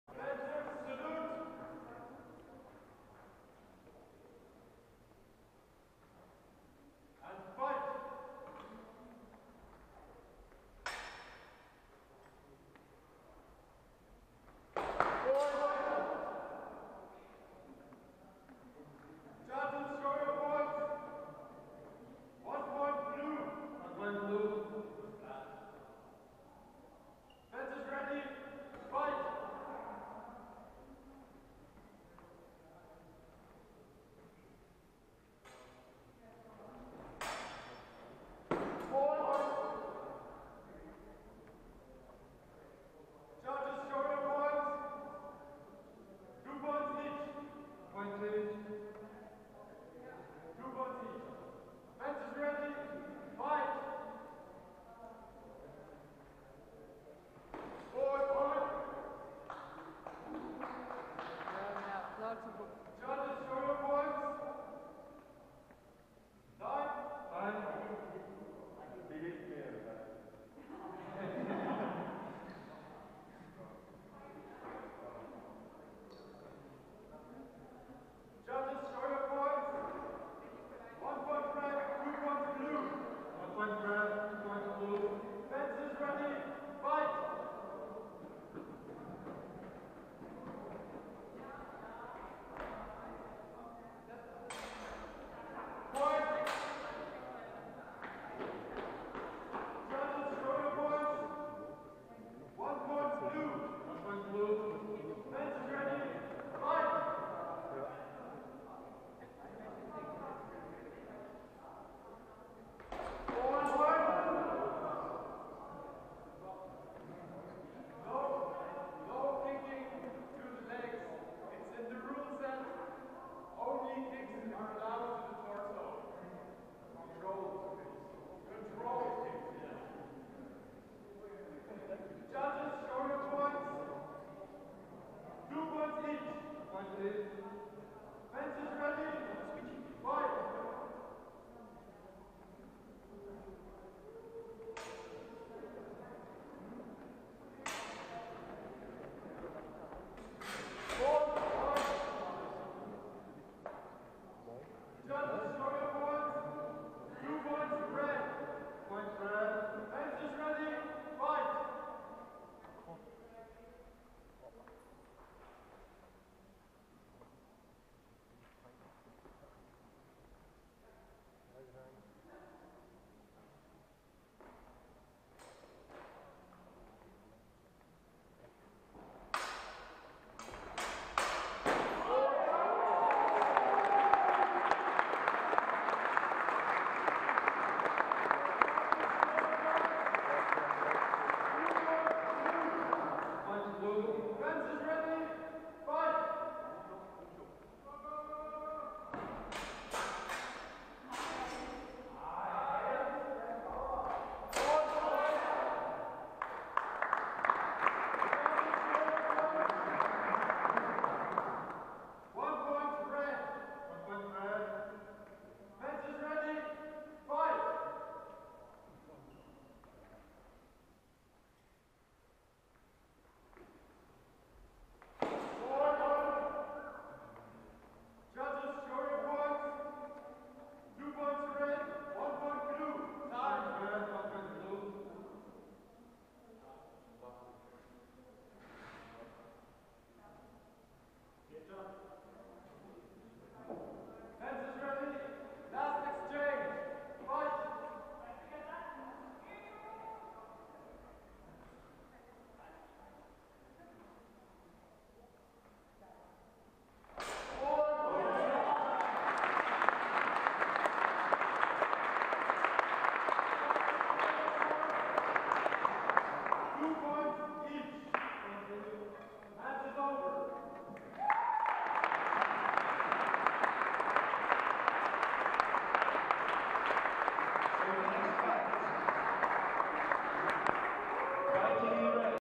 Swiss Bears Cup 2020 - Octofinals - Nicolai Ismail Bauer vs. Sandro Kämpf
Nicolai Ismail Bauer (INDES Salzburg, AUT, blue) vs. Sandro Kämpf (Säbelrassler, SUI, red)